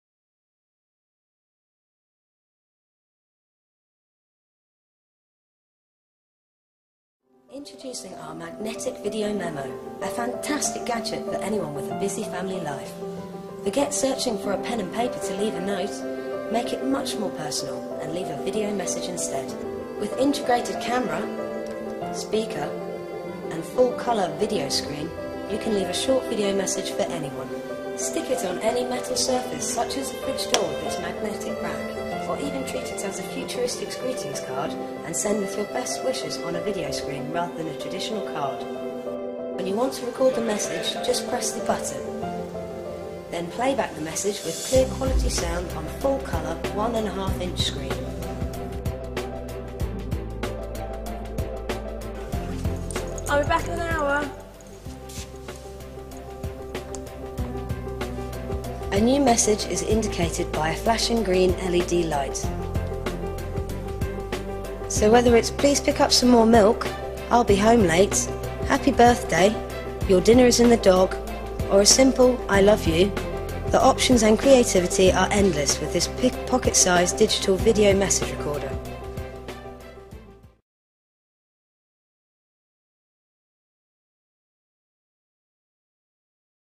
Digital Video Memo
Forget about leaving vague and confusing handwritten notes with refrigerator magnets. This clever video fridge magnet allows you to record clear and concise video memos that's sure to get the point across in 30 seconds.